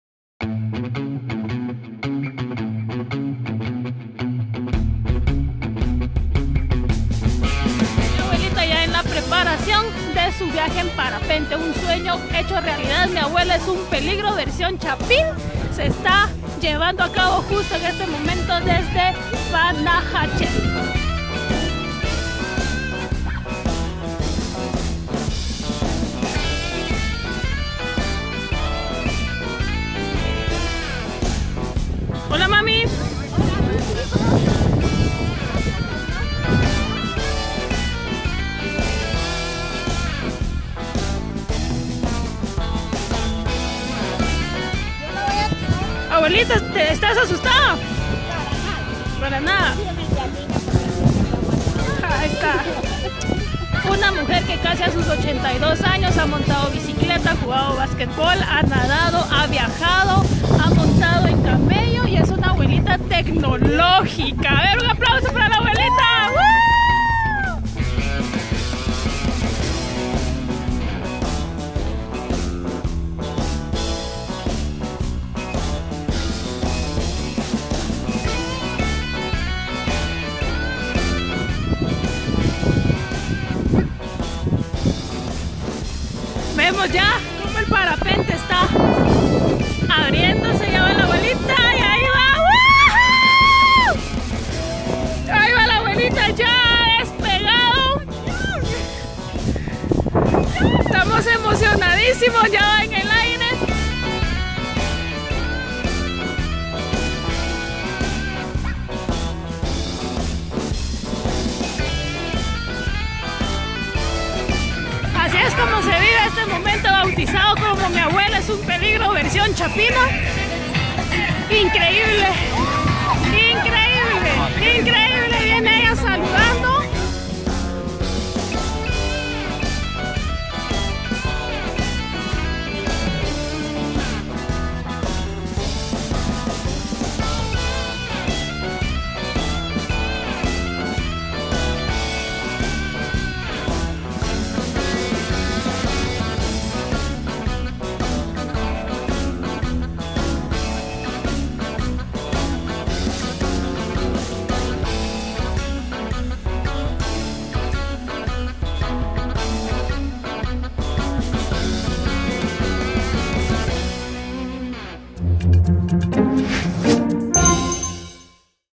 Una abuela en parapente
Alicia De Arévalo de 81 años cumplió unos de sus sueños, volar en parapente. Su familia compartió los videos de su vuelo. (Prensa Libre: Facebook/ Eu LoAre)